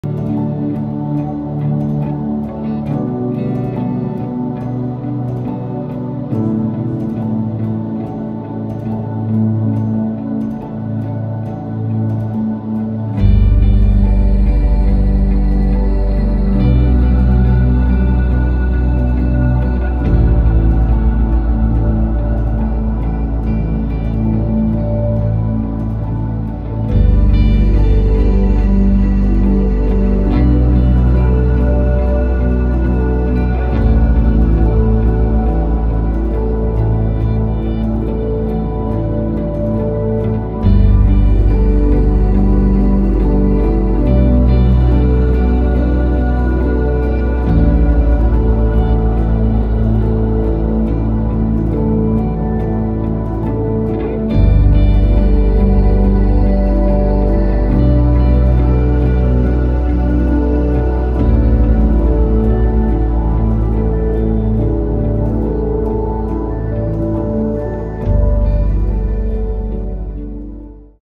Cambridge on Film
Another evening walk in Cambridge city center and an attempt to play around with color grading.